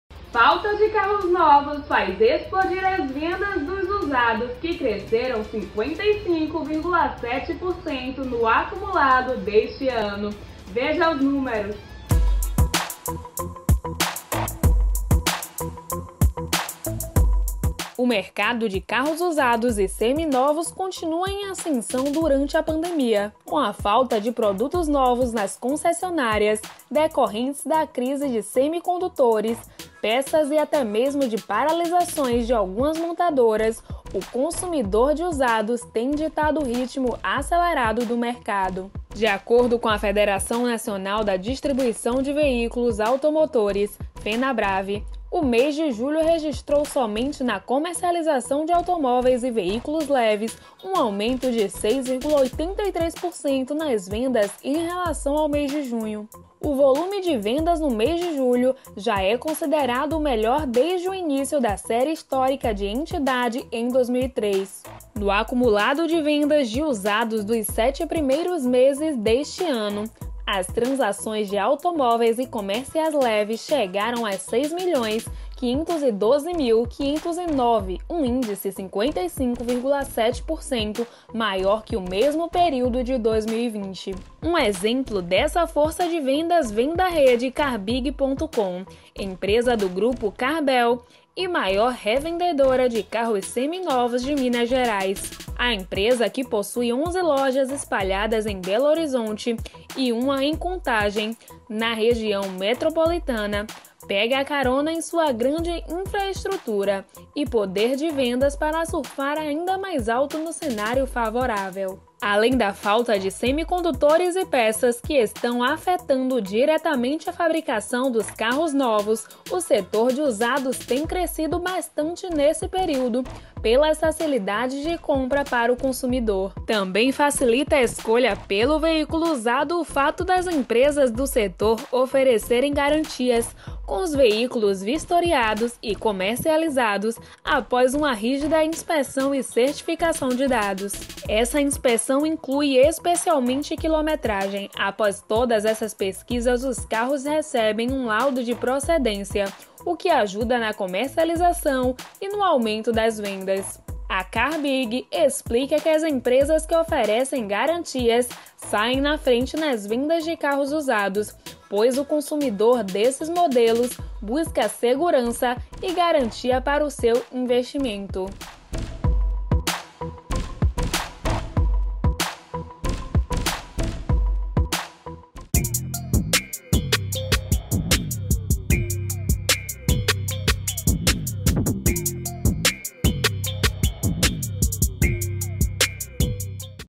Explodem as vendas de carros usados no Brasil
Falta de carros novos faz explodir as vendas dos usados, que cresceram 55,7%, no acumulado deste ano. Veja os números.//